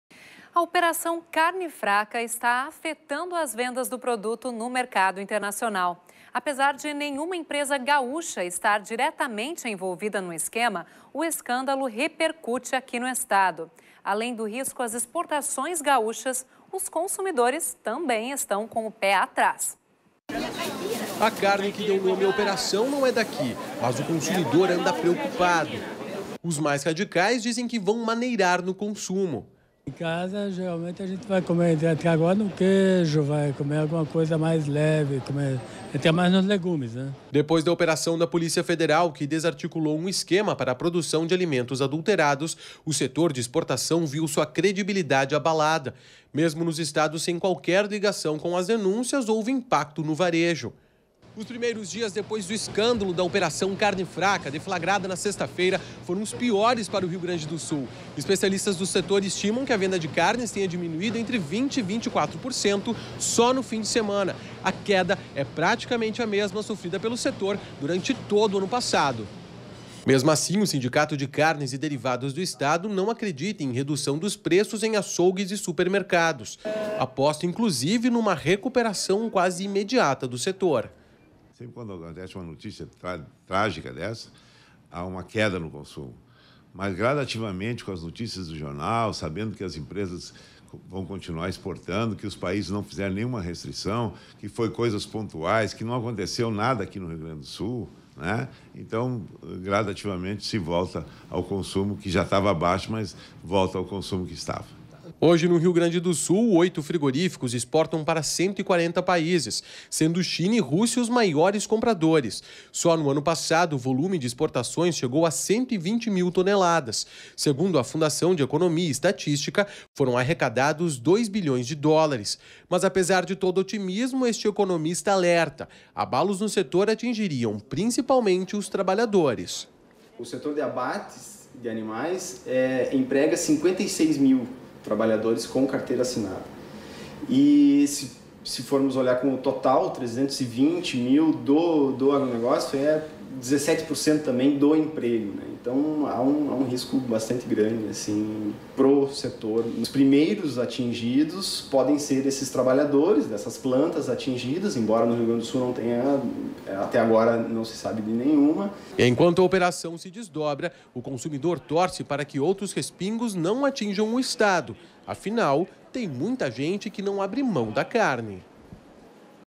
A Operação Carne Fraca está afetando as vendas do produto no mercado internacional
A Operação Carne Fraca está afetando as vendas do produto no mercado internacional. Apesar de nenhuma empresa gaúcha estar diretamente envolvida no esquema, o escândalo repercute aqui no estado./ além do risco às exportações gaúchas, os consumidores também estão com o pé atrás.